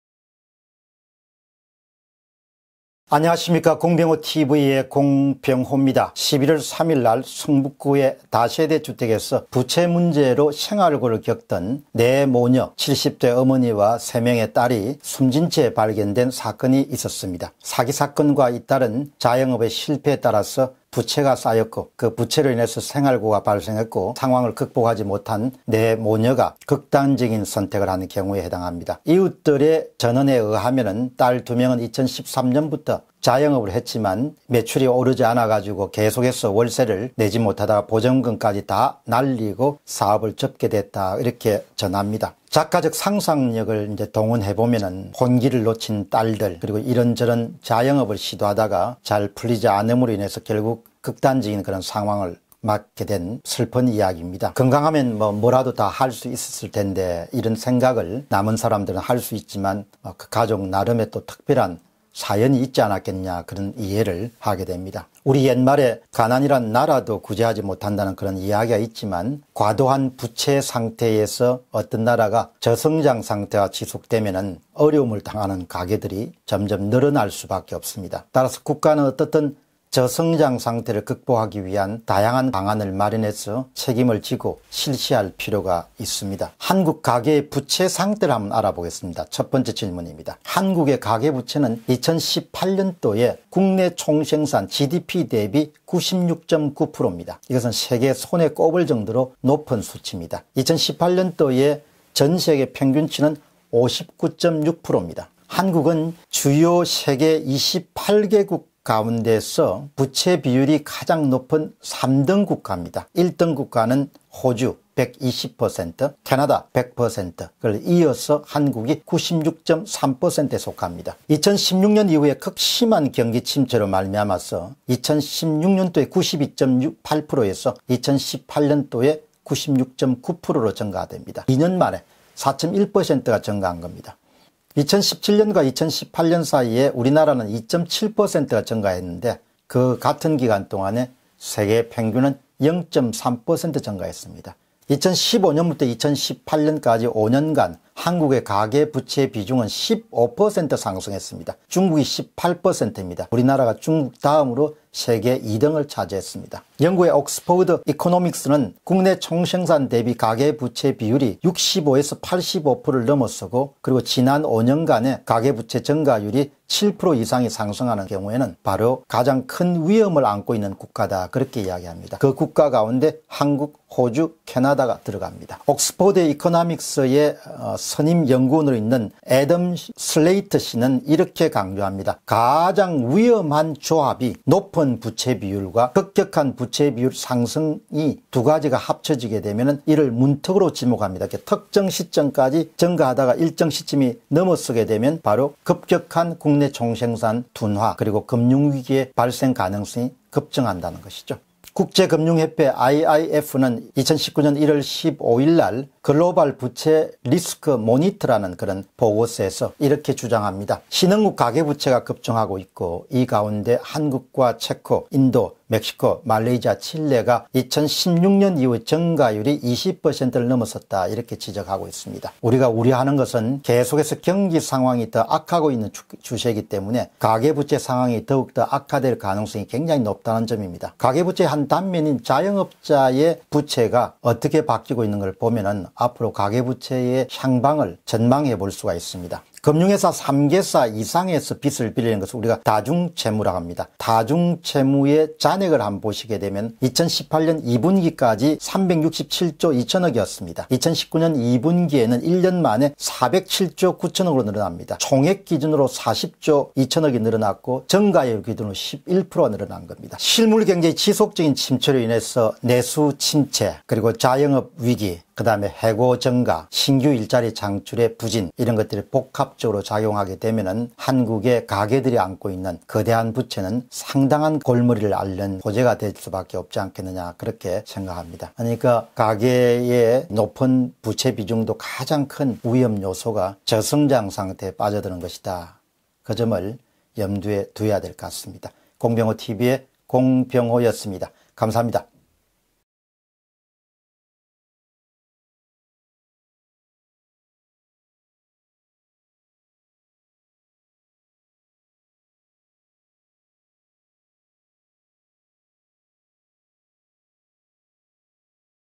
세계 3위 한국 / 가계 부채의 미래 [공병호TV]
세계 3위 한국
가계 부채의 미래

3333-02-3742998 카카오은행 (예금주 : 공병호) / (텔레뱅킹 : 090 & 은행코드 : 90)

참고자료
1. 성북구서 70·40대 네 모녀 숨진 채 발견…‘하늘나라로 간다’ 유서 남겨, 김소영 이소연, 동아일보, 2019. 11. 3
2. ‘옥천 네 모녀 사망 사건’ 남편 의식 회복…“가족 살해 진술”,오윤주, 한겨레,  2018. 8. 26
3. 한국 가계부채 증가속도 세계 2위 ... GDP 비율은 최고, 연합뉴스, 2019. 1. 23
4. "대출 받아 버티는 식당…옆집 사장은 카드론 10개 돌려막기", 강인선, 매일경제, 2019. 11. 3